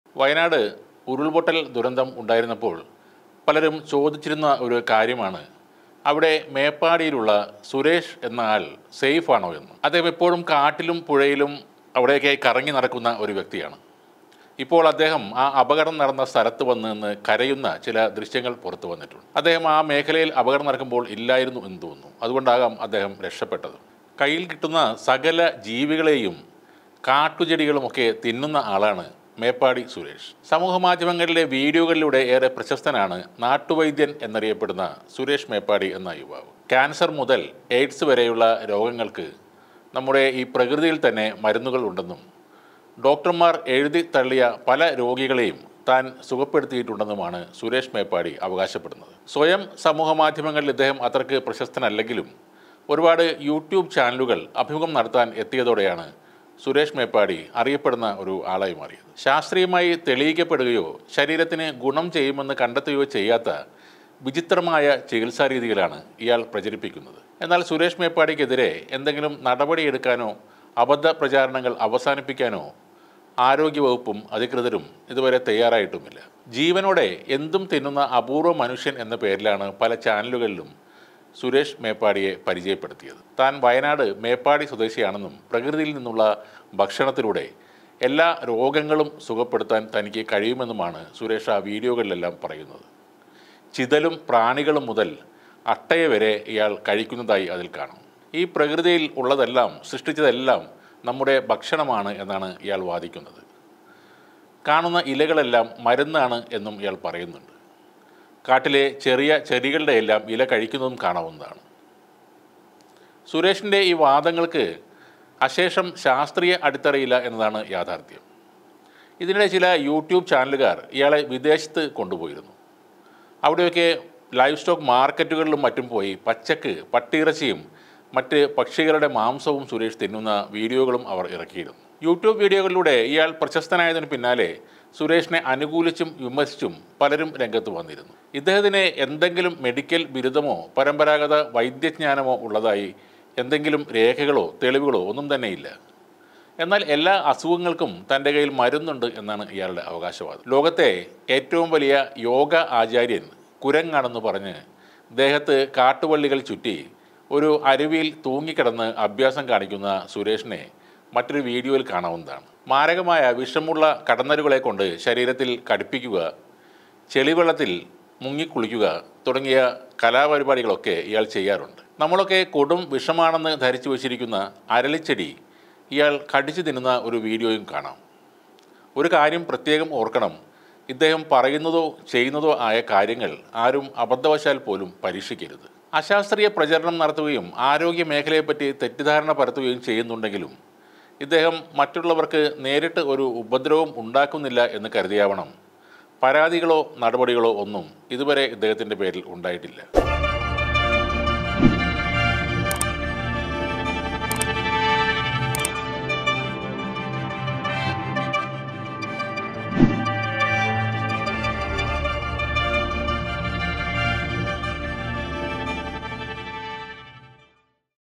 ആനപ്പിണ്ടം, പല്ലി, പാറ്റ, പാമ്പ്, ചാണകം തുടങ്ങി പട്ടിക്കാട്ടം വരെ പച്ചക്ക് തിന്നുന്ന മനുഷ്യൻ;
ആനപ്പിണ്ടം, പല്ലി, പാറ്റ, പാമ്പ്, ചാണകം തുടങ്ങി പട്ടിക്കാട്ടം വരെ പച്ചക്ക്  തിന്നുന്ന മനുഷ്യൻ; ദുരന്തഭൂമിയിൽ പൊട്ടിക്കരയുന്ന സുരേഷ് മേപ്പാടി